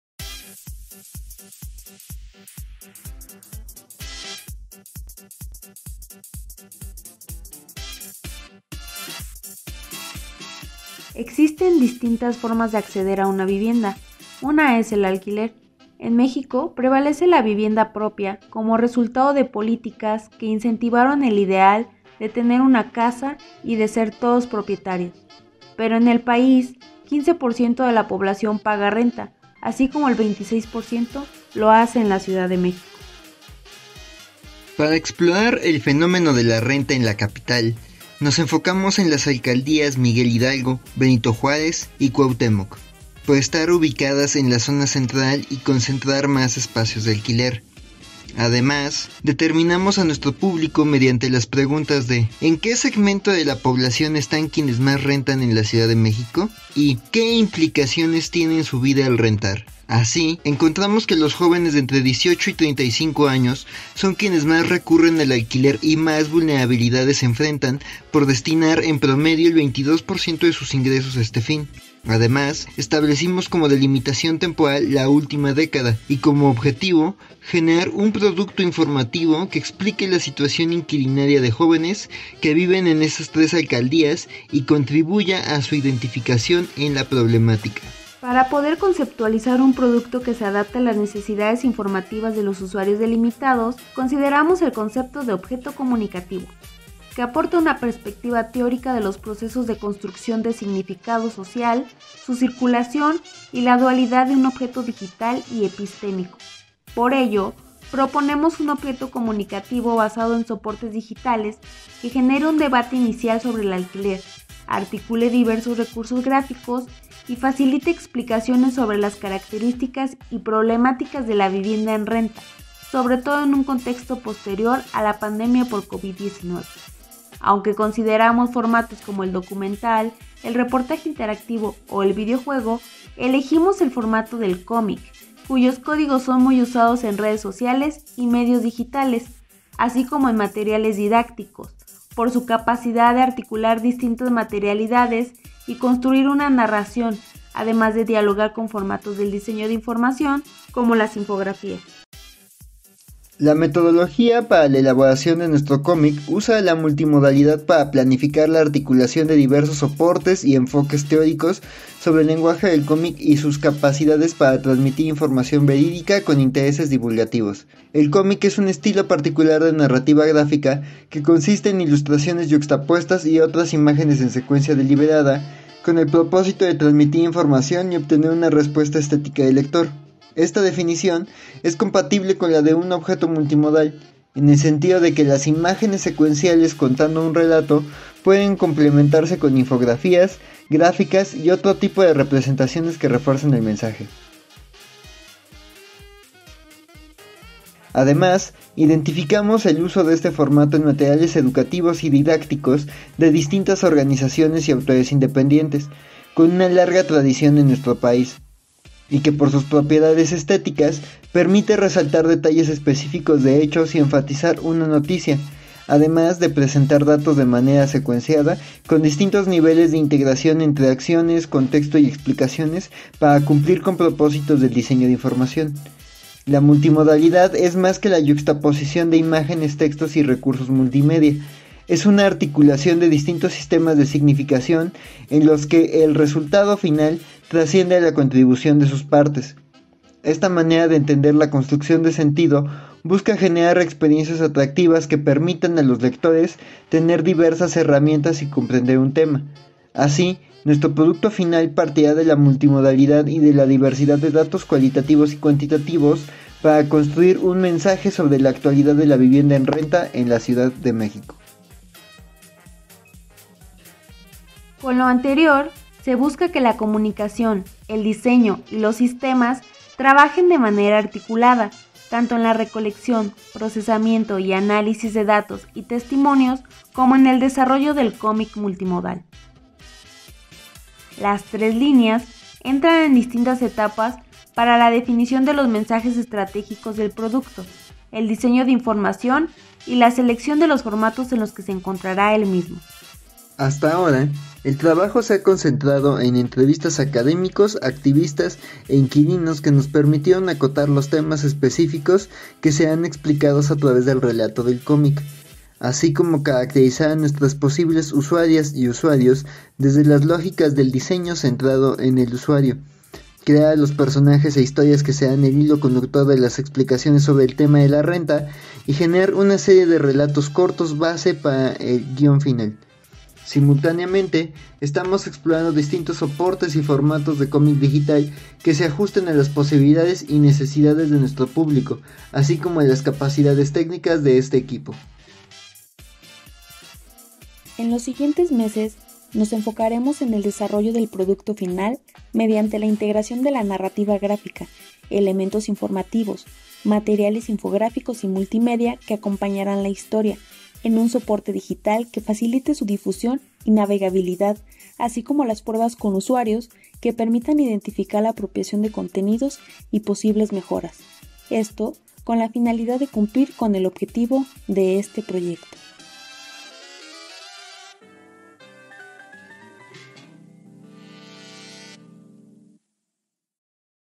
Cómic multimodal para explicar la situación inquilinaria
Proyecto Terminal.
Maestría en Diseño, Información y Comunicación MADIC.

Cómic multimodal para explicar la situación inquilinaria de jóvenes en tres alcaldías de la Ciudad de México

Alumnos:
Ricardo Axel Alonso García y Alma Delia Sánchez Carbajal

Asesor:
Dr. Raúl Roydeen García Aguilar